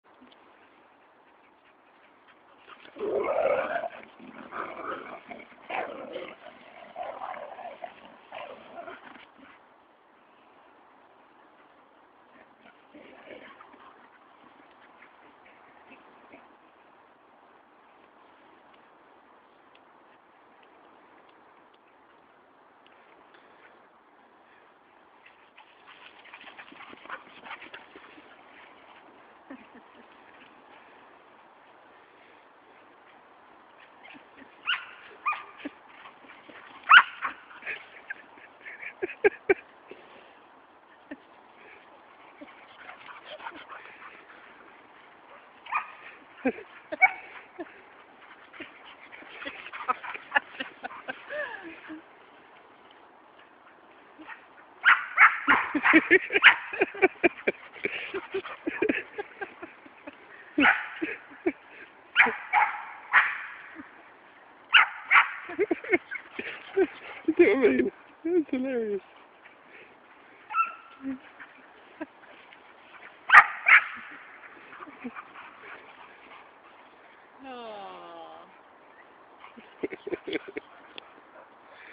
Two dogs going around and around and around....
Murphy our springer spaniel is chasing Teasel, our parents german short haired pointer through the woods to get this ball back!...